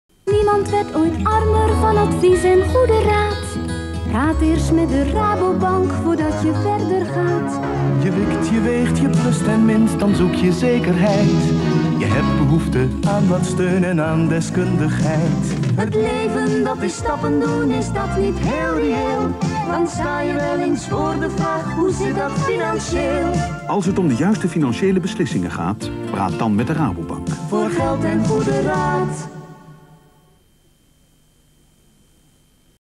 TV: Rabobank - Vakantie, Huis Kopen, Gezin (Voor Geld en Goede Raad) (1987) | Reclame | Commercial
Rabobank reclamespot uit 1987.

Met dank aan Esther voor het beschikbaar stellen van deze videoband.
Source: Vide 2000, PAL.